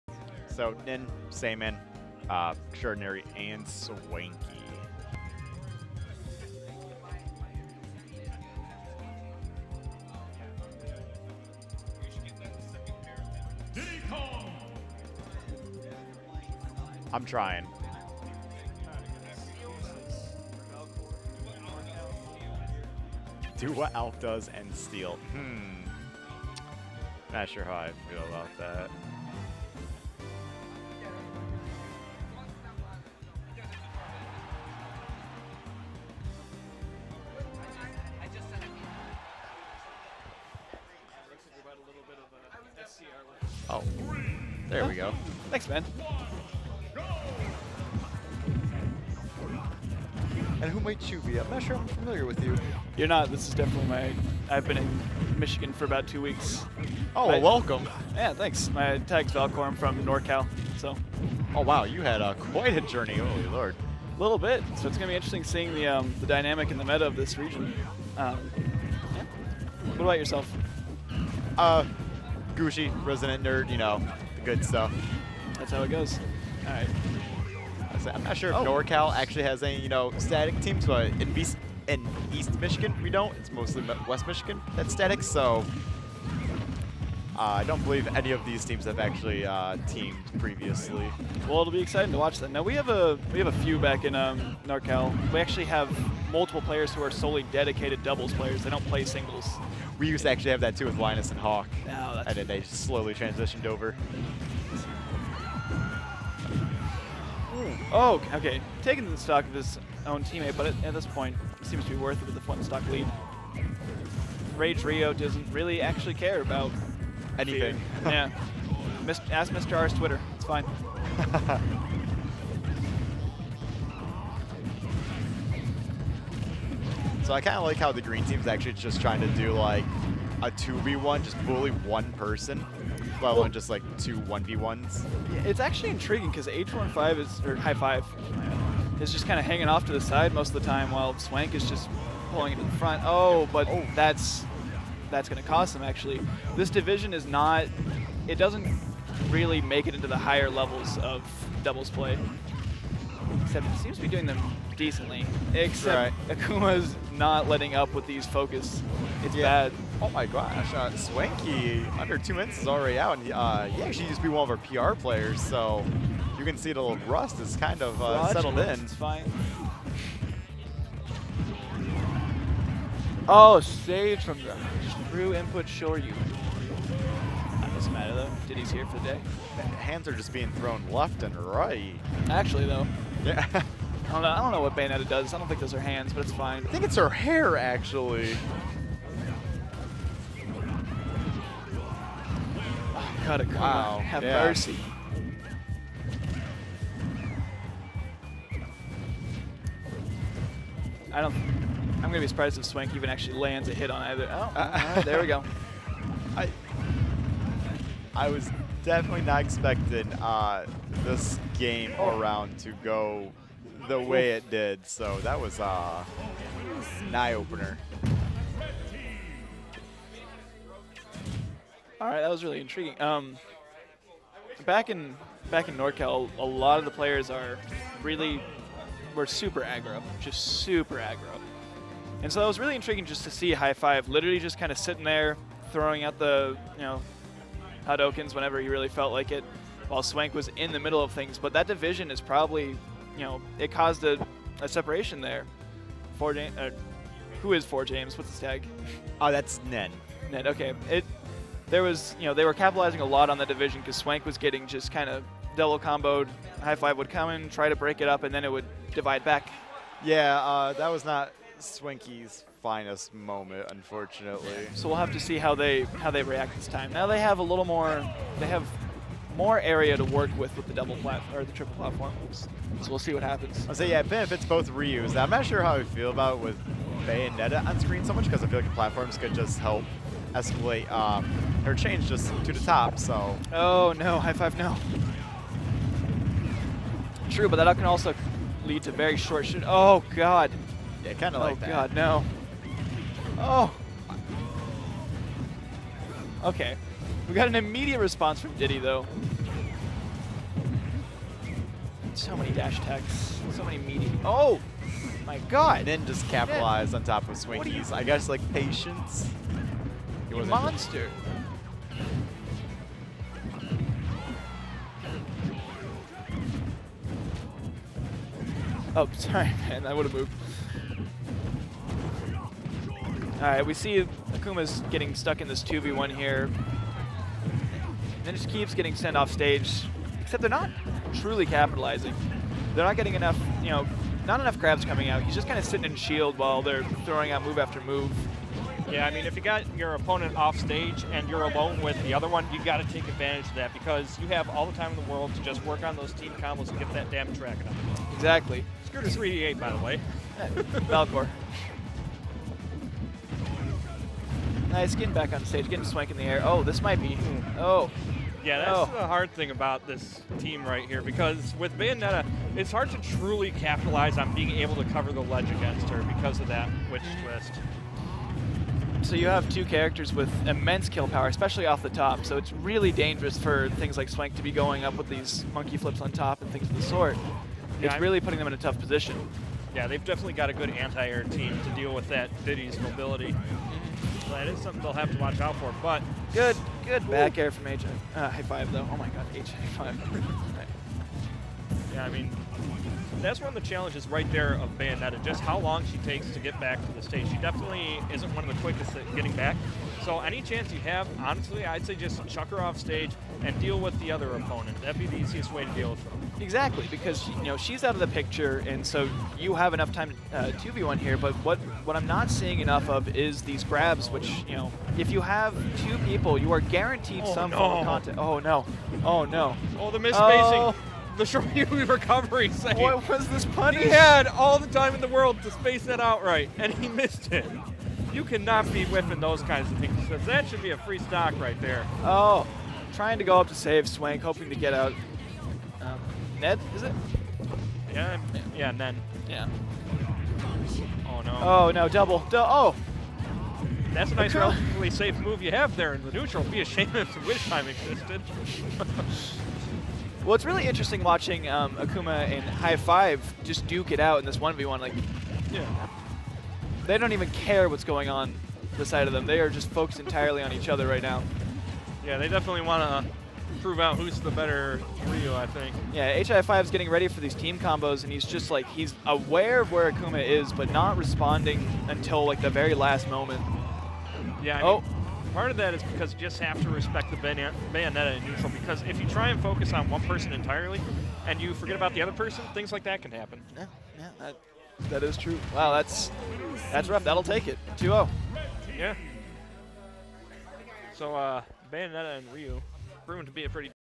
SFW #104: Nen/Saiyaman (Bayo/Ryu) vs exJORDANary/Swanky (Ryu/Diddy)
SFW #104 (Doubles) Winners R1
8/24/17
Troy, MI

Bracket: Smashfield.challonge.com/SFW104

Support GooshiGaming at:

Twitch.tv/GooshiGaming
Facebook.com/Gooshigaming
Twitter.com/GooshiGaming